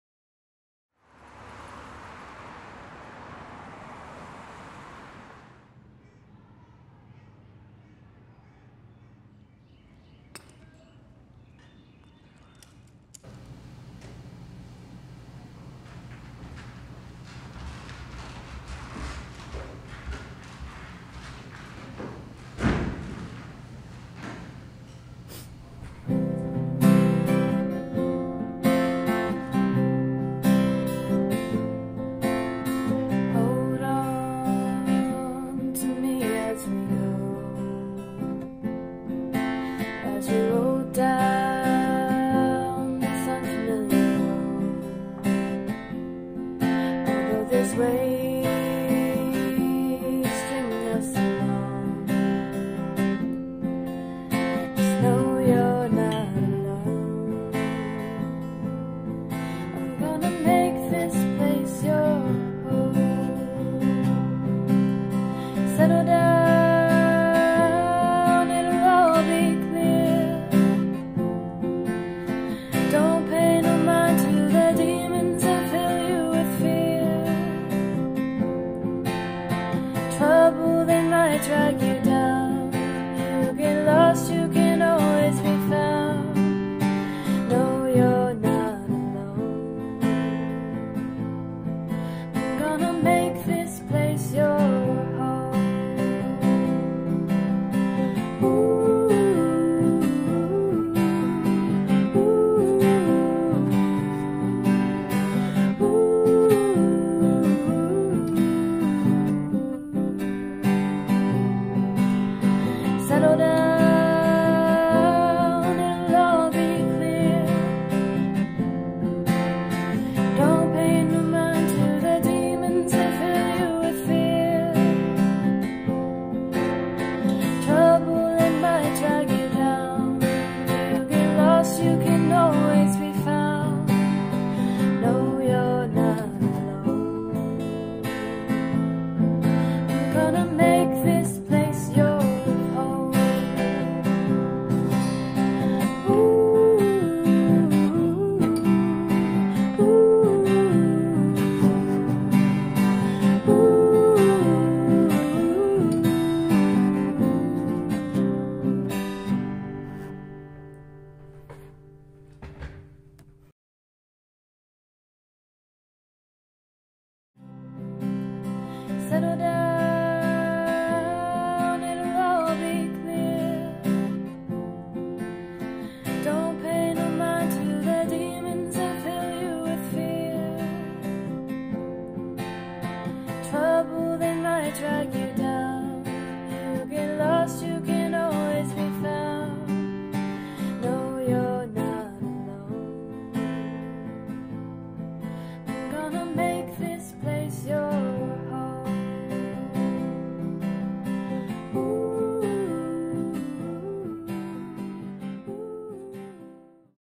UCSCA 2017
A video compilation showcasing a week at the best summer camp on earth.